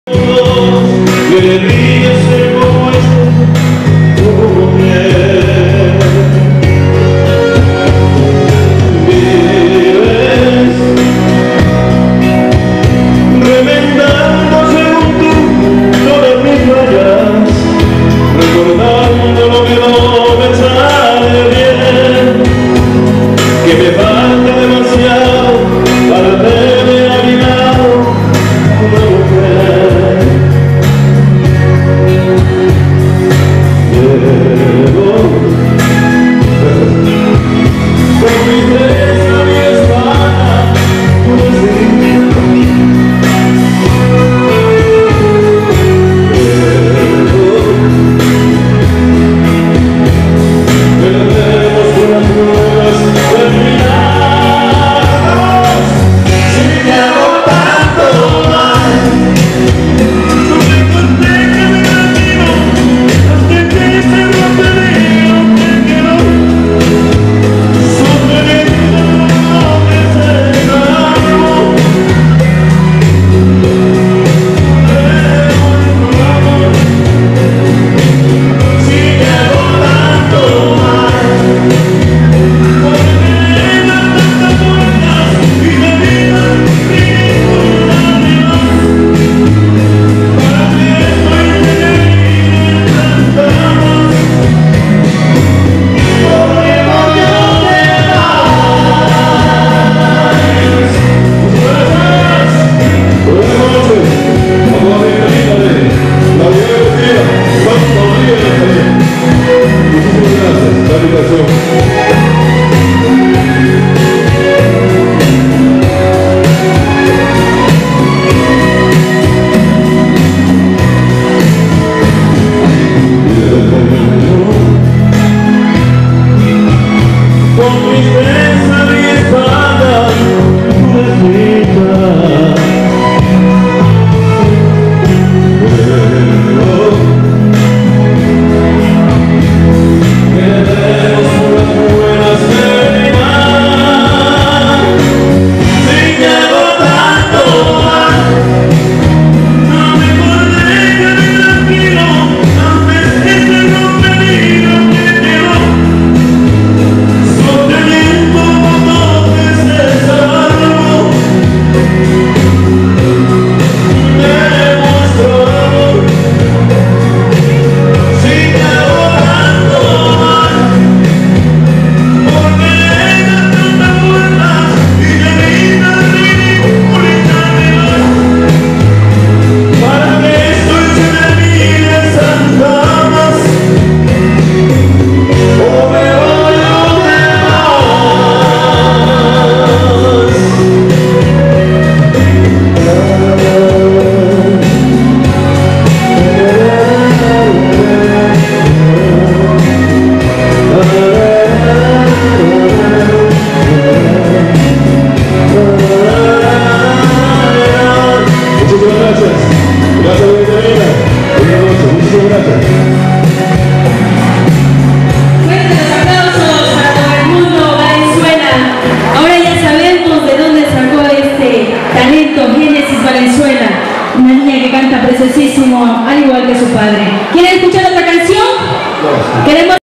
Edmundo Valenzuela
Presentación de Edmundo Valenzuela, en la elección y coronación de la señorita Nazareth 2017.
Foto Díaz 2 presente, opoyando el talento de La Unión.